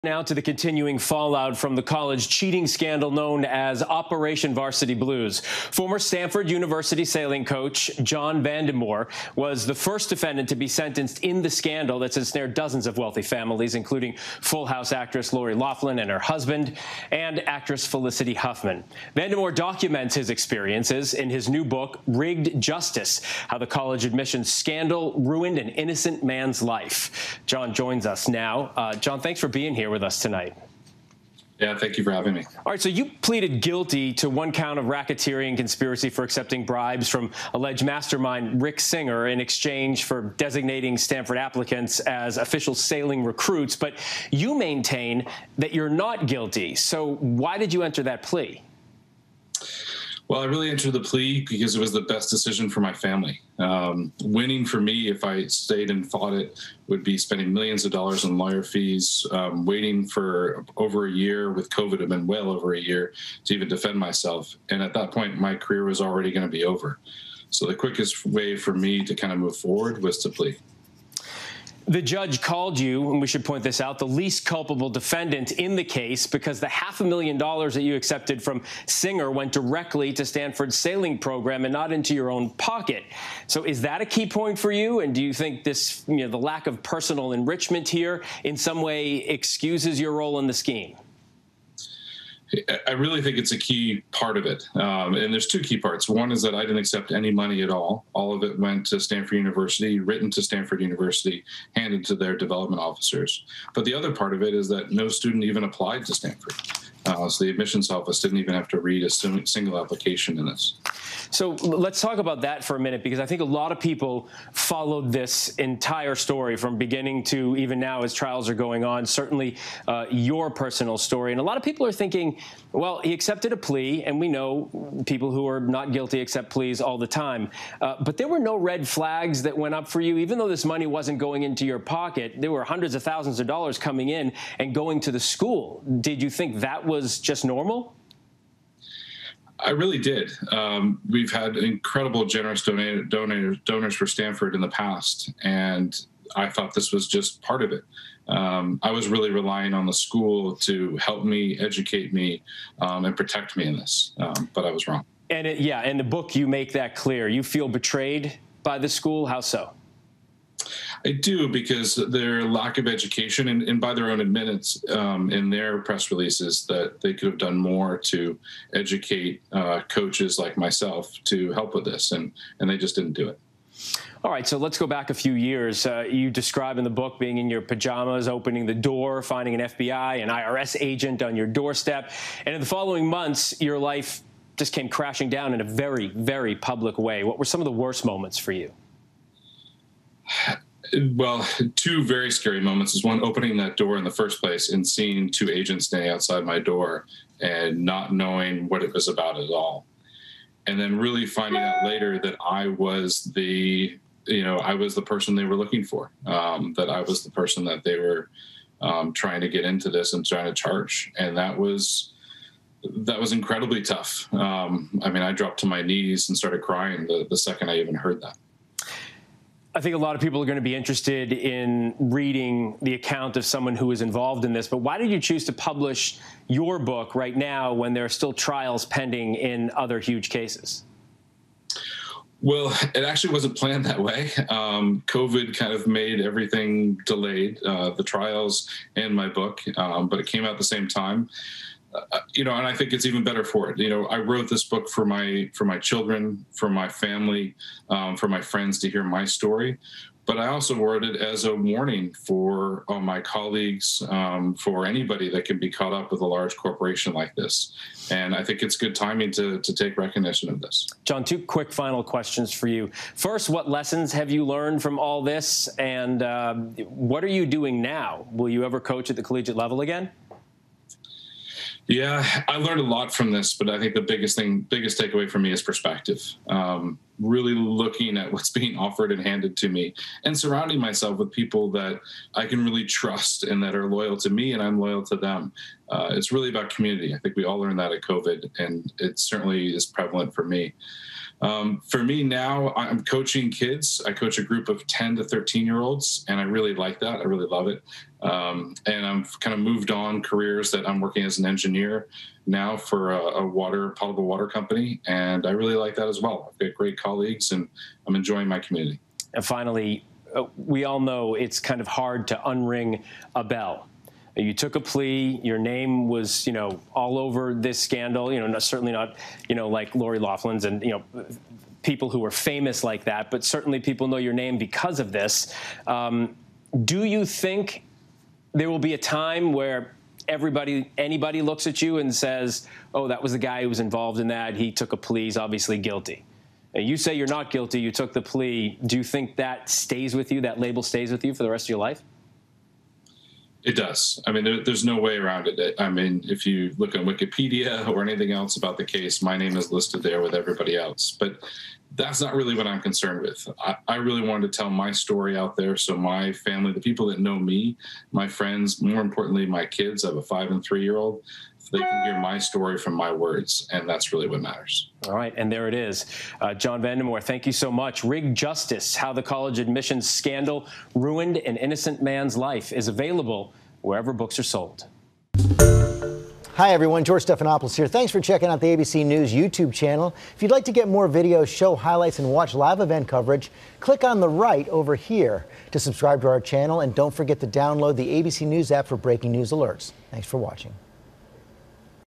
'Varsity Blues' scandal defendant pleads guilty because 'it was best for my family'
ABC News’ Phil Lipof speaks with former Stanford sailing coach John Vandemoer, author of the new book “Rigged Justice,” on his conviction in the “Varsity Blues” college admissions scandal.